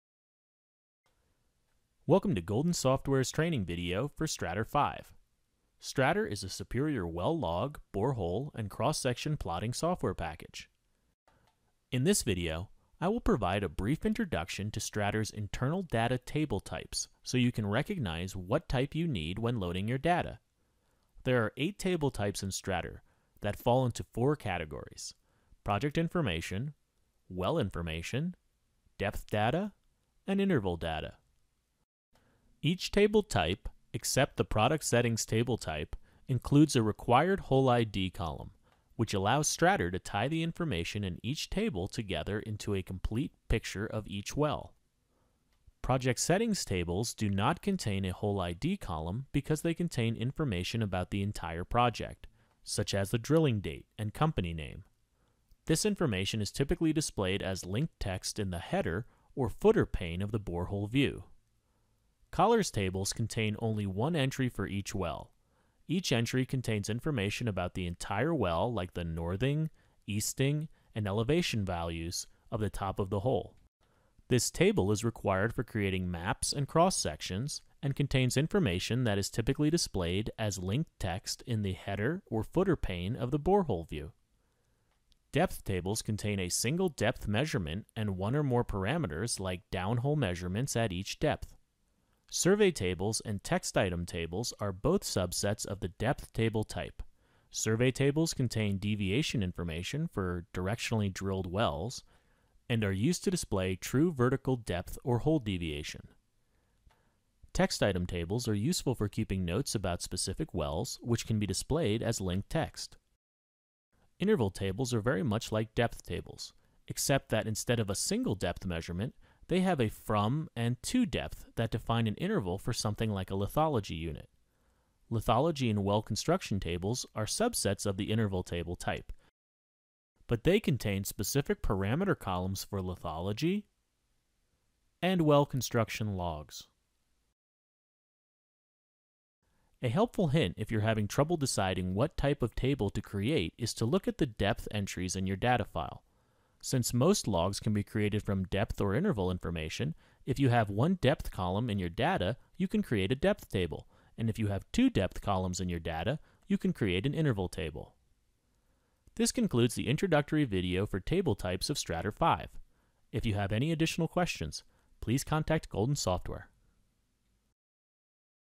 Strater 5 Table Types
This video introduces the 8 internal data table types of Strater so you can make an informed decision about what type to load your data into.